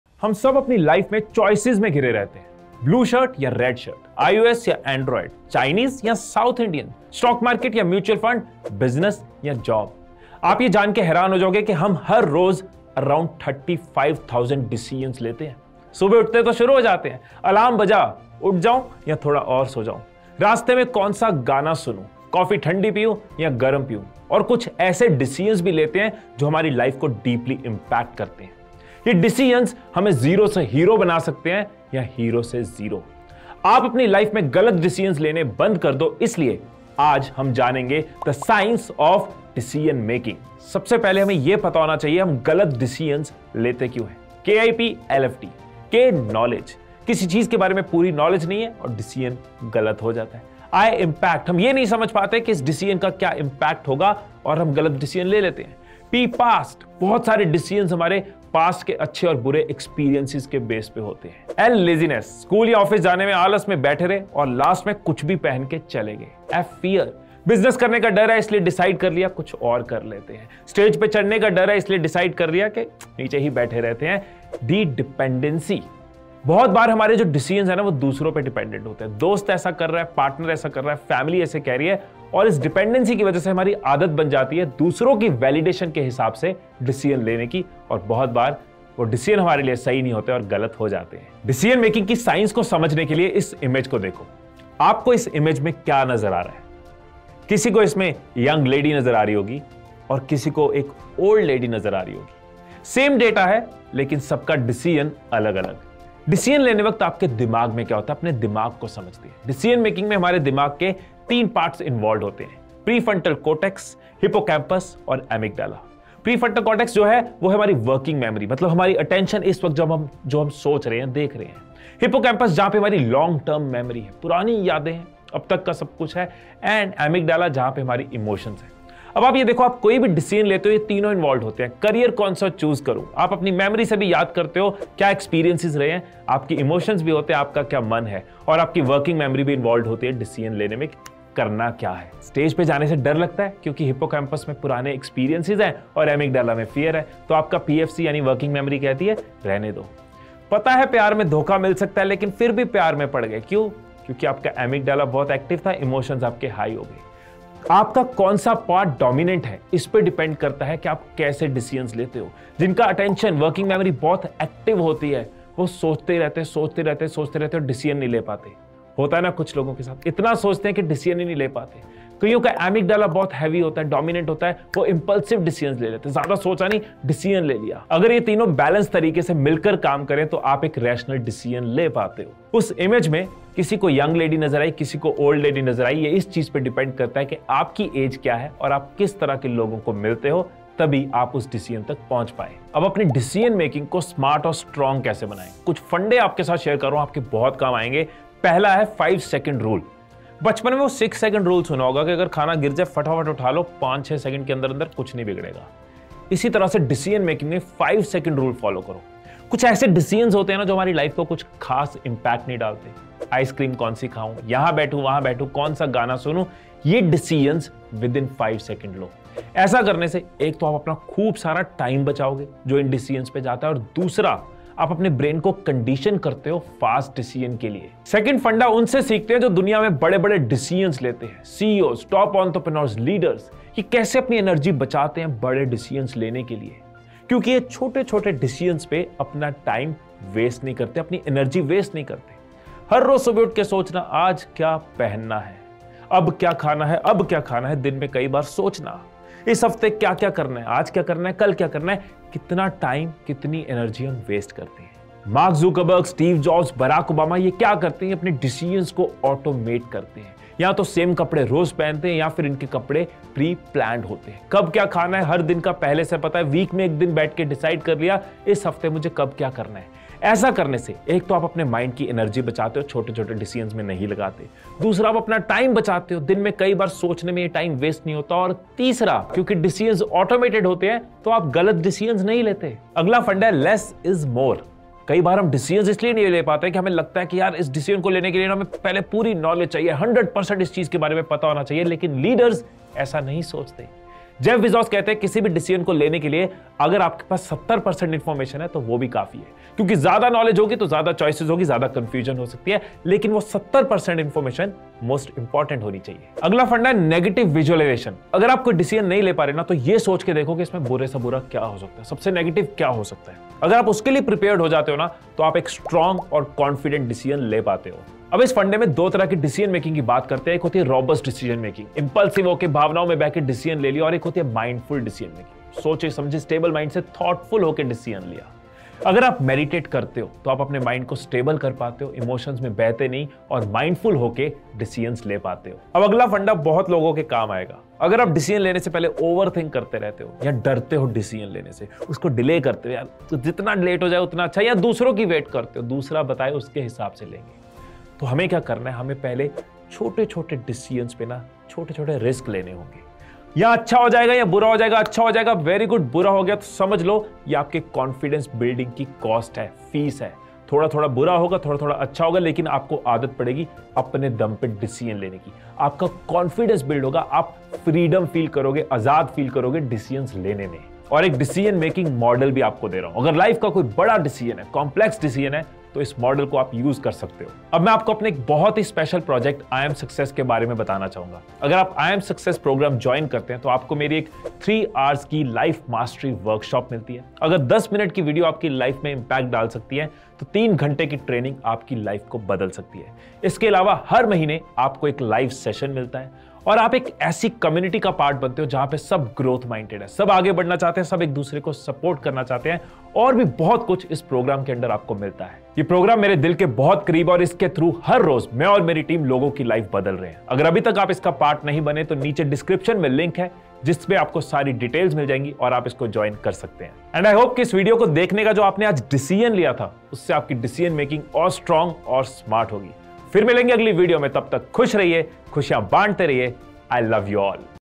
How to take Right Decisions? by Him eesh Madaan
Learn Smart Decision making and Science behind it !!

This video is divided into 3 PartsFirst part in this video talks about Why we take wrong Decision in Life as we can not solve the problem without understanding the root cause of confusions and wrong decisions.
Second part talks about Science behind our Decisions, how our brain functions when it comes to taking Decisions. How our short term and long term memory along with our emotions play an important role in our Decision making.

Third Part talks about some tips and steps which you can take to make right decisions in life which will help you to grow faster in Life and become more successful in your career and Life.